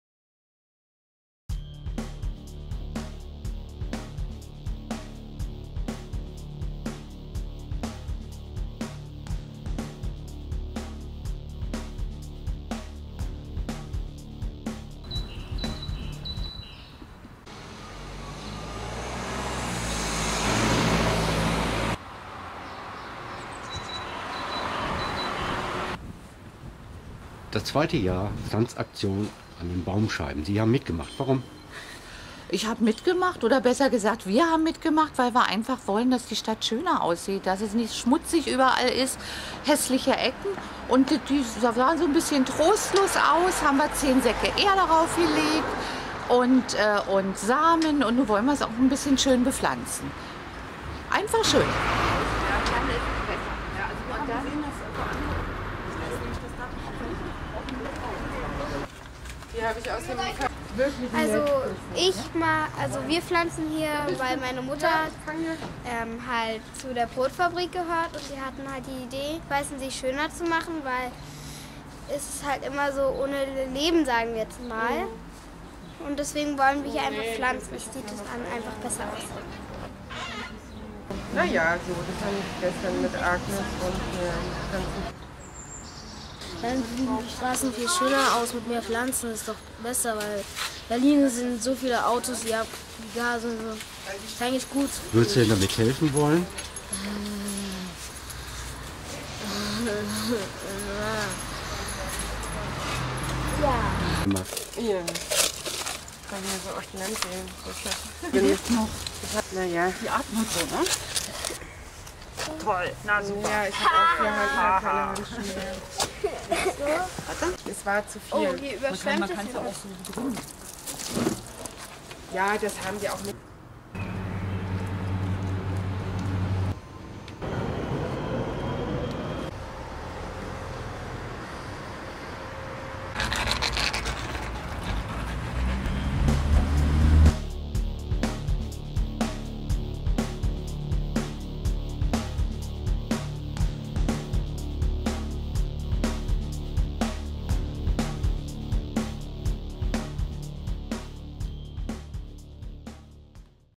Baumscheibenbepflanzung am 9. April 2011 in Berlin-Weissensee
Auch in diesem Frühjahr startete die Baumscheibenbepflanzung in Weissensee. Bürger_innen gestalten ihren Kiez mit neuen Pflanzen.